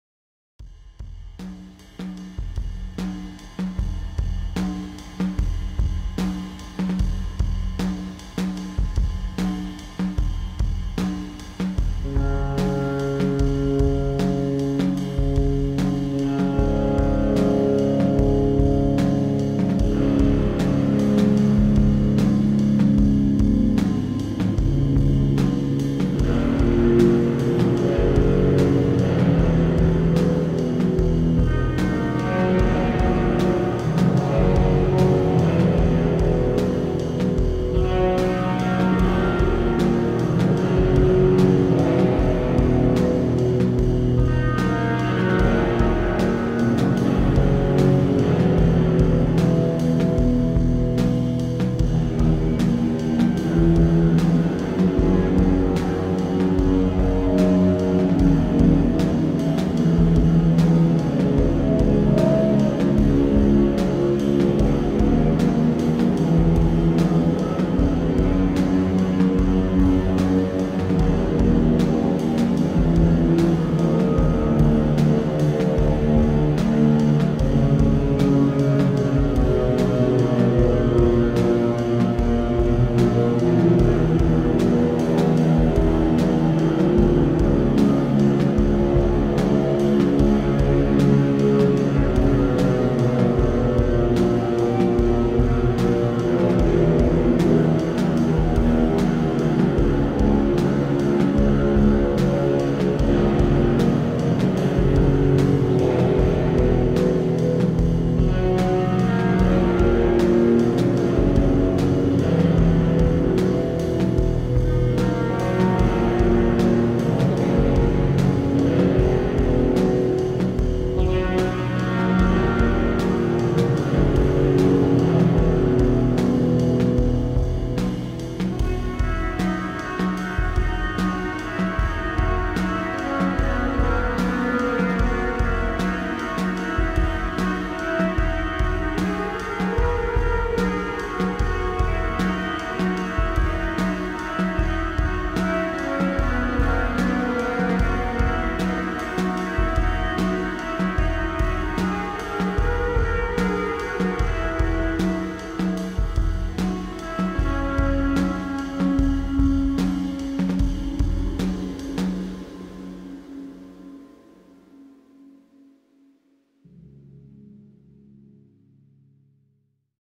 Ambient inspired by Nikita Kryukov's "Milk outside a bag of milk outside a bag of milk"
Guitar: Washburn WR150
Daw: Reaper
USB audio interface: FOCUSRITE Scarlett Solo 3rd Gen
VST: Guitar Rig 6
Tuning: C# G# C# F# A# D#